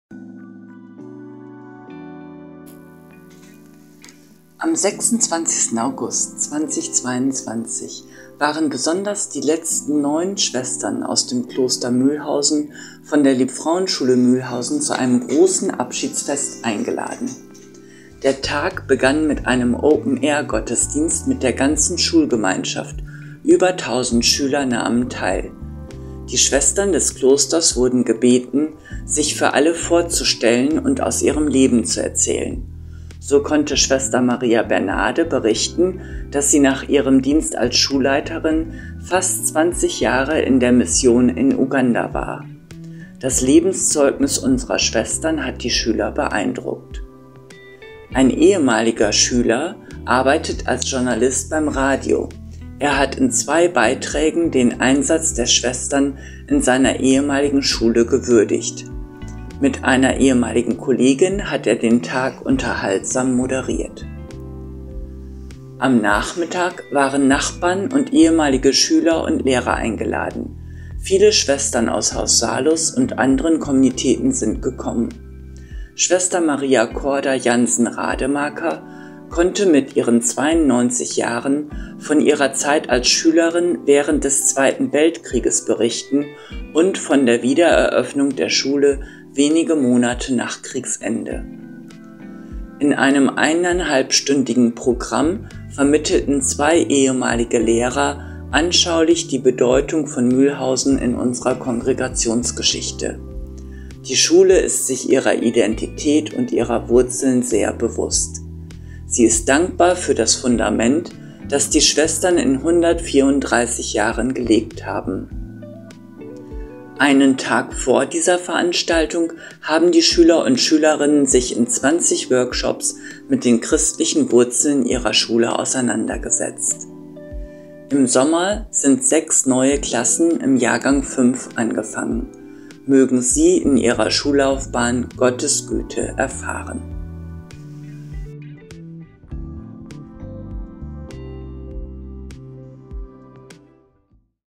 Verabschiedung der Schwestern von Mülhausen durch die Liebfrauenschule
Am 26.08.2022 waren besonders  die letzten 9 Schwestern aus dem Kloster Mülhausen von der Liebfrauenschule Mülhausen zu einem großen Abschiedsfest eingeladen.

Der Tag begann mit einem Open-Air- Gottesdienst mit der ganzen Schulgemeinschaft, über 1000 Schüler nahmen teil. Die Schwestern des Klosters wurden gebeten, sich für alle vorzustellen und aus ihrem Leben zu erzählen. So konnte Sr. M. Bernarde berichten, dass sie nach ihrem Dienst als Schulleiterin fast 20 Jahre in der Mission in Uganda war. Das Lebenszeugnis unserer Schwestern hat die Schüler beeindruckt.